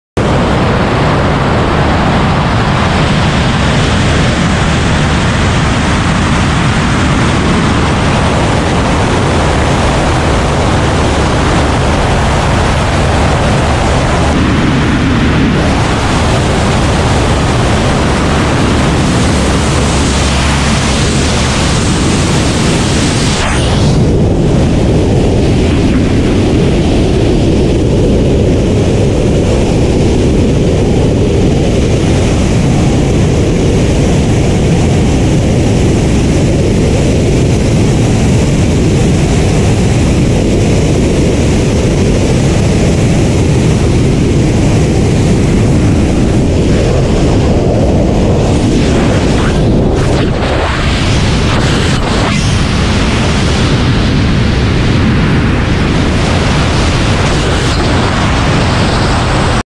Skydiving alongside plane
A guy wearing a wingsuit skydiving alongside a plane in free fall.